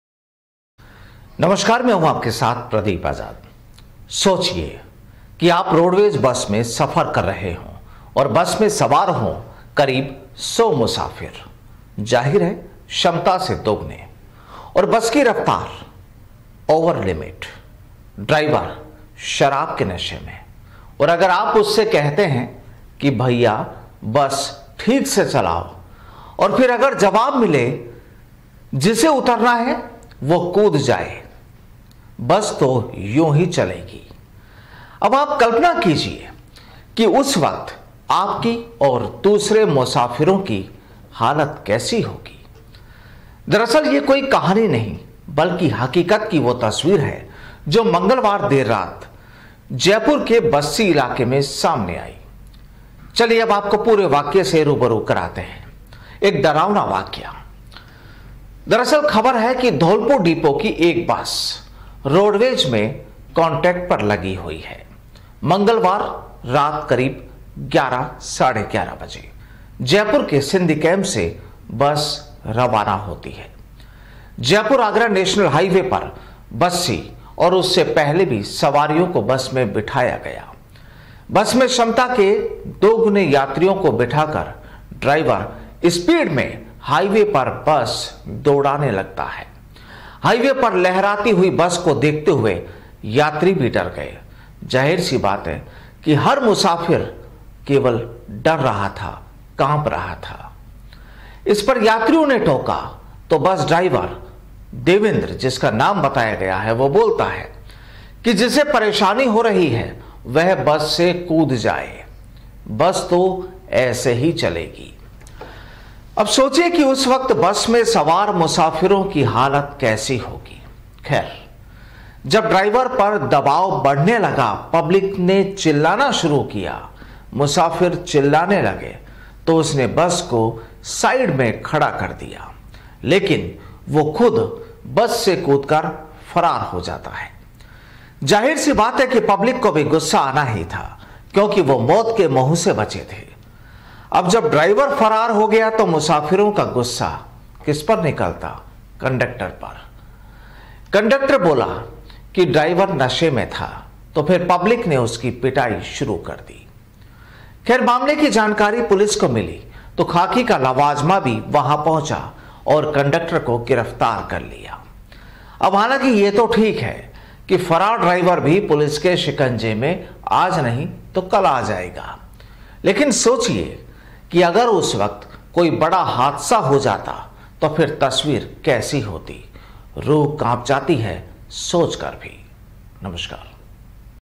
100 Passengers की जिंदगी से खेल रहा था ये Bus Driver! || RSRTC || Jaipur Viral News
100 Passengers की जिंदगी से खेल रहा था ये Bus Driver! || RSRTC || Jaipur Viral News ||